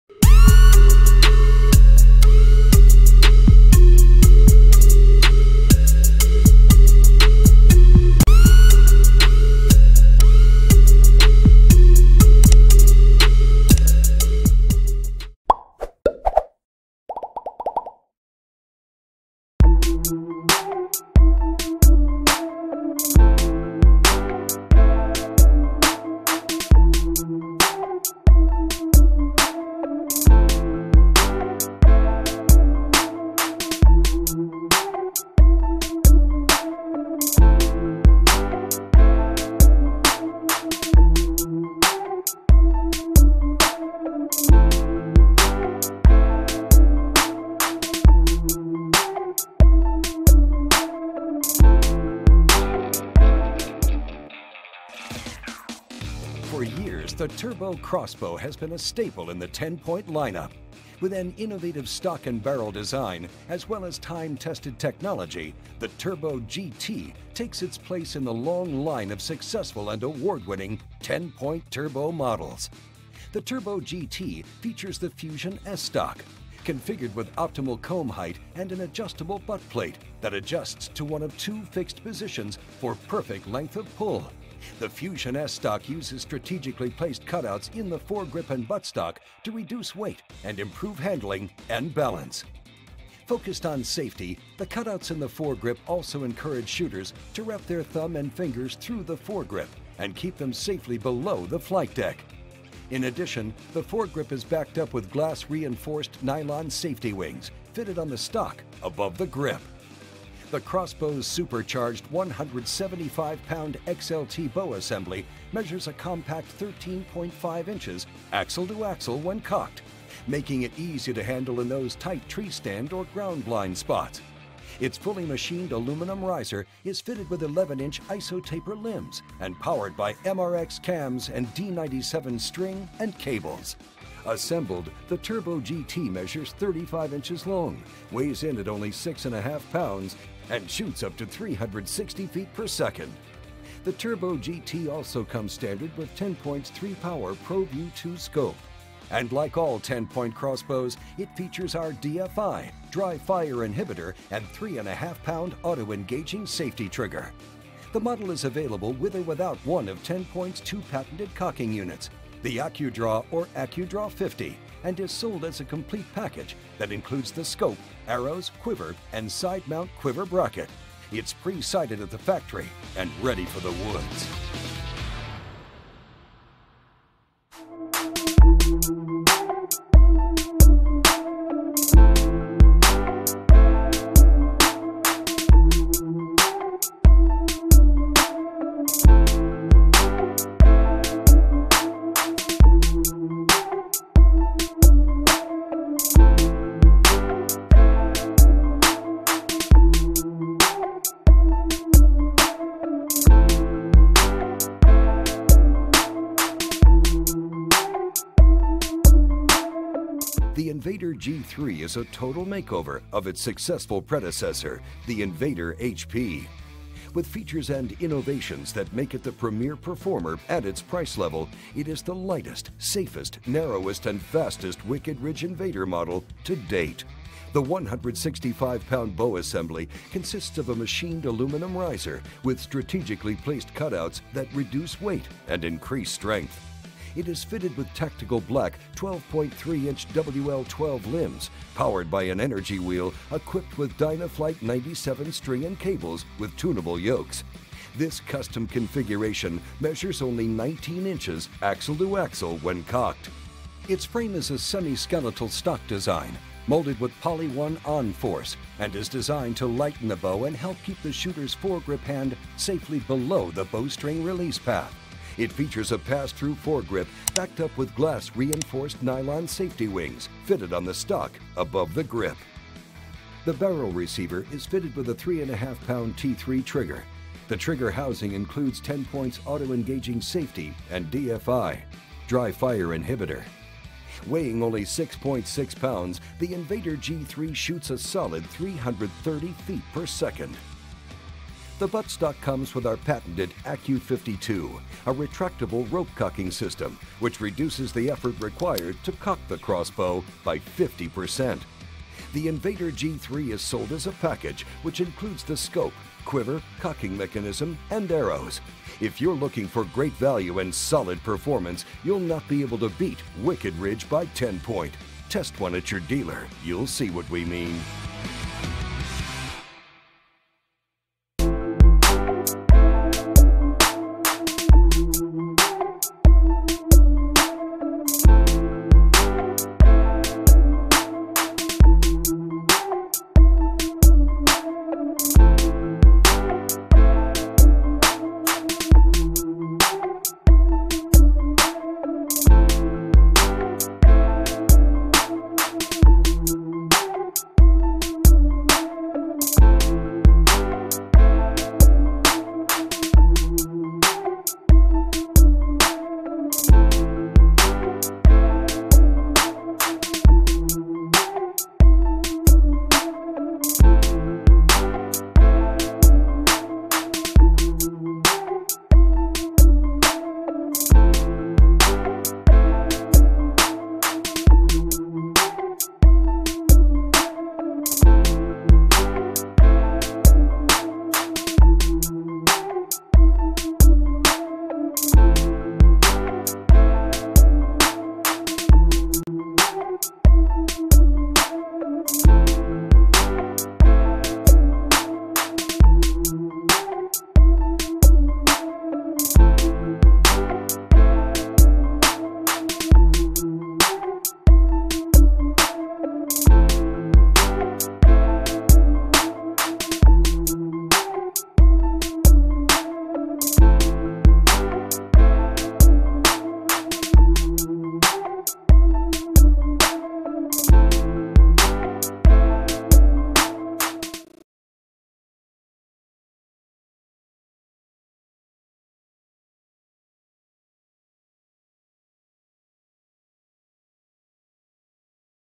Most Powerful Crossbow in the World - 5 Best Crossbow
Links to these Best Crossbow mentioned in this video:

1. Tenpoint Turbo GT Crossbow
2. TenPoint Invader G3 Crossbow
3. CenterPoint Sniper 370- Crossbow
4. Bruin Ambush 370 Crossbow
5. XtremepowerUS Multifunctional